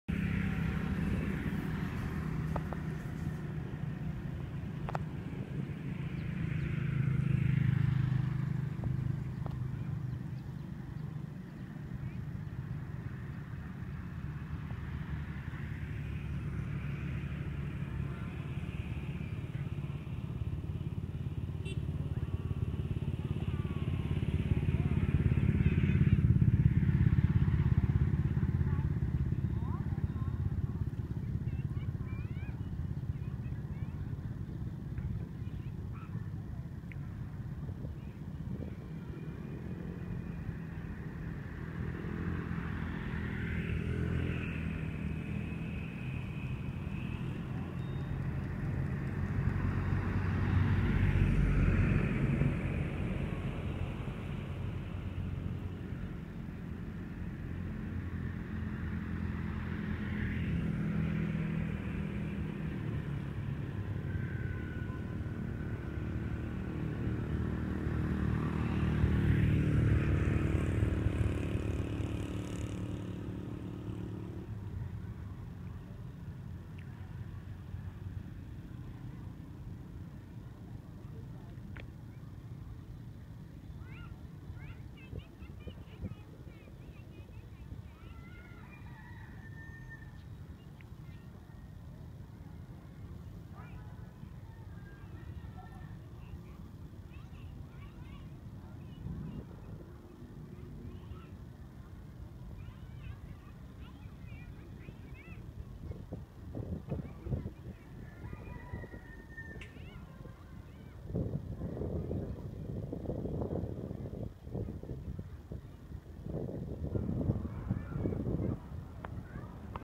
Mây Ngũ Sắc - Phần 3
Quay trên đường đến Cảnh Bình Phú
2022.06:06 17:54
Phạm vi công cộng